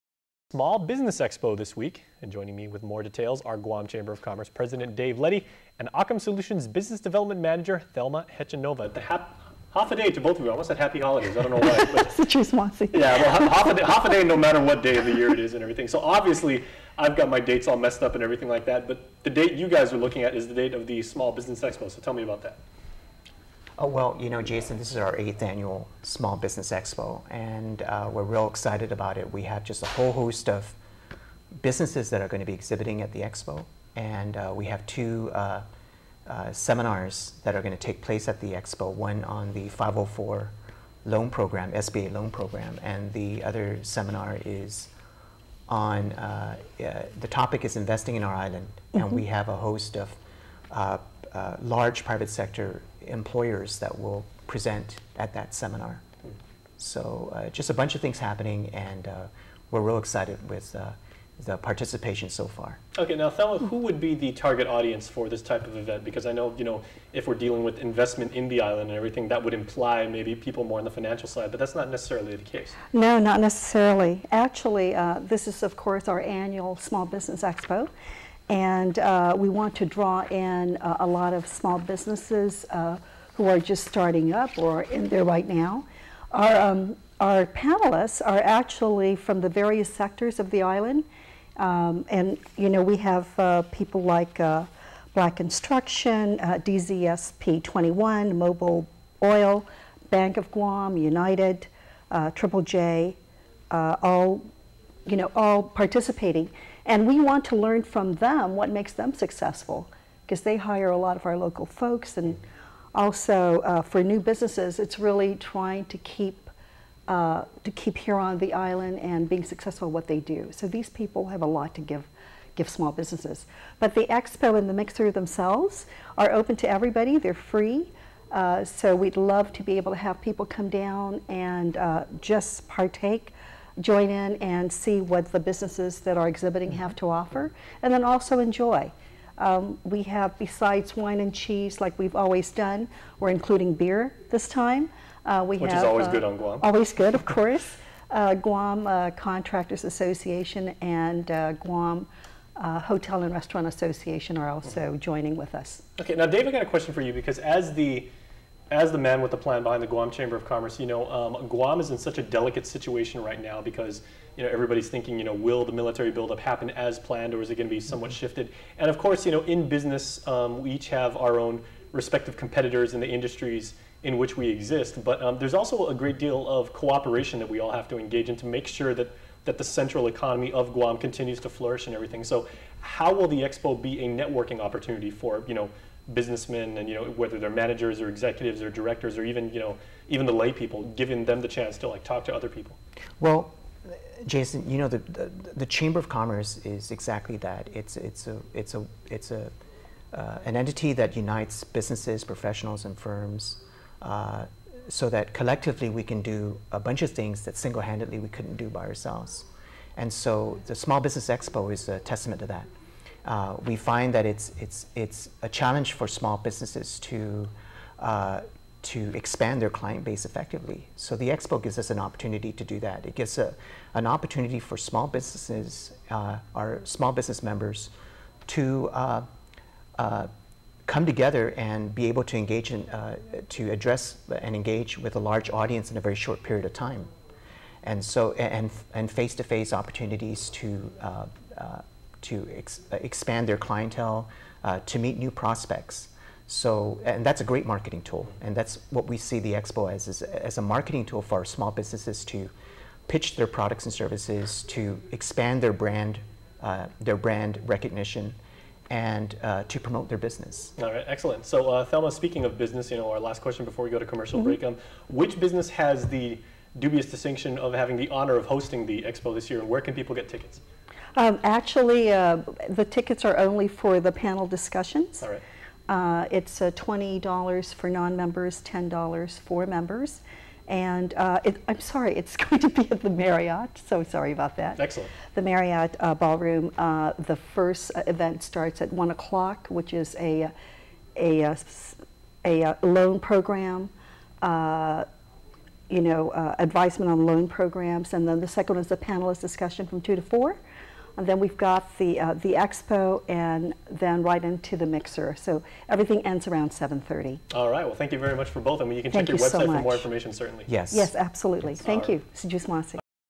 Ideas, innovation and networking at the Guam Small Business Expo
KUAM's Jason Salas talks to Guam Chamber of Commerce president Dave Leddy about the Guam Small Business Expo.